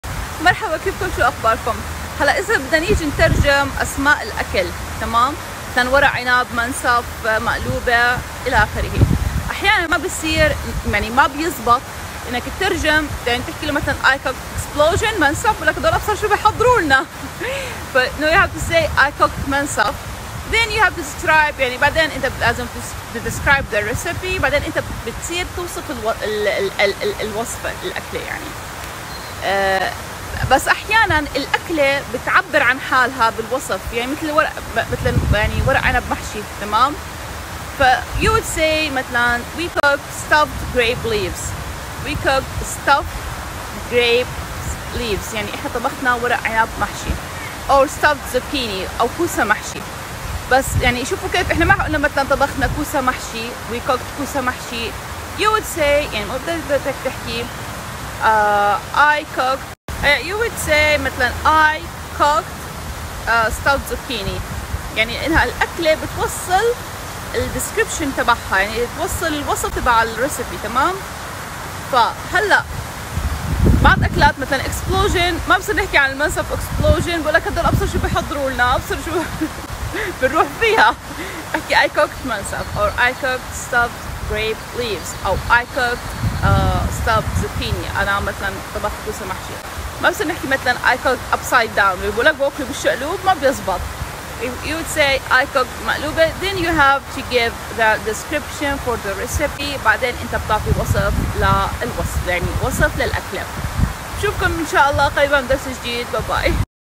معنى ورق عنب بالانجليزي تحدث_الانجليزية_من_الصفر الانجليزية _العامة
I cooked stuffed grape leaves.
I cooked stuffed zucchini.

هذا الدرس يحتوي على جمل وعبارات بالانجليزي نستخدمها بشهر رمضان عن ترجمة اسماء الاكلات مثل ورق عنب محشي بالانجليزي او كوسا محشي بالانجليزي. و اتمنى تشتركوا بالقناة حتى تبدا تعلم الانجليزية من الصفر و خاصة عن الانجليزية بالحياه اليومية،

عنوان صفحتي على الفيسبوك

اعداد اسلام الشبار
تحدث الانجليزية مع اكاديمية الشبار